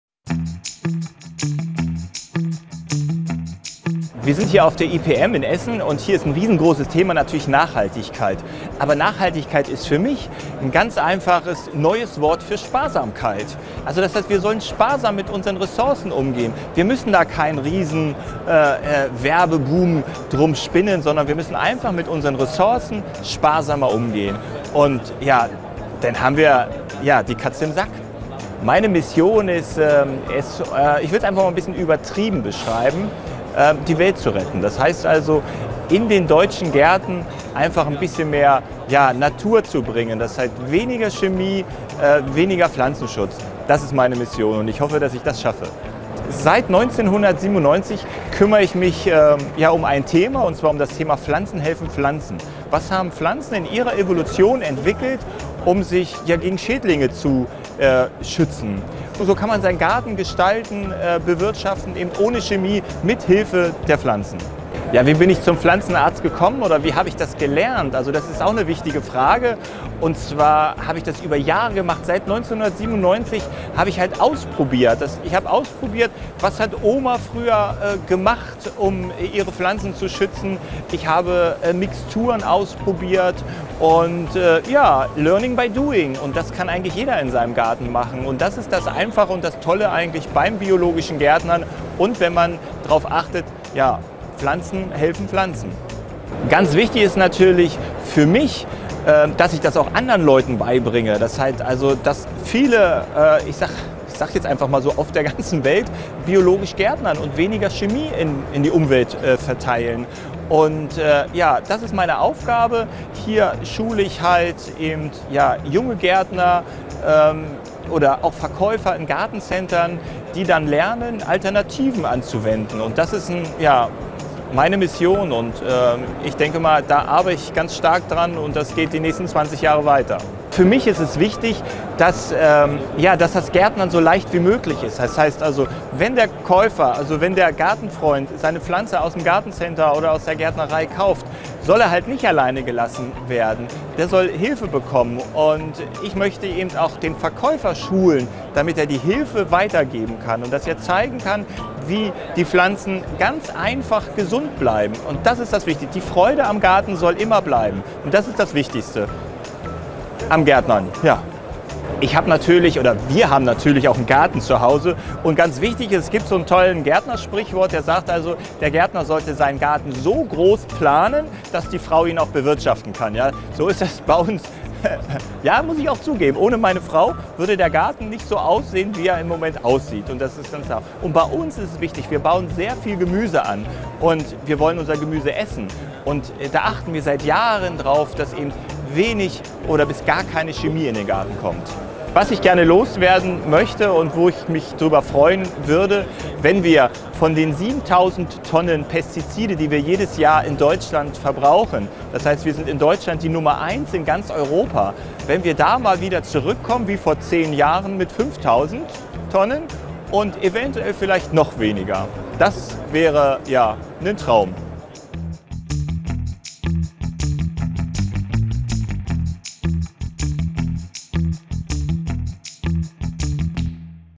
GARTEN TV ShortTalk: Der Pflanzenarzt - Die Mission des René Wadas
Auf der IPM - der Weltleitmesse des Gartenbaus - in Essen habe ich Pflanzenarzt René Wadas getroffen. Was ist seine Mission? Was hält er als Gärtner und Naturschützer vom "Nachhaltigkeits-Hype"? Hat er eine Vision? Vielen Dank an René Wadas für das Gespräch! Eine ALL-IN-ONE (Eigen)Prodouktion von Anne Welsing.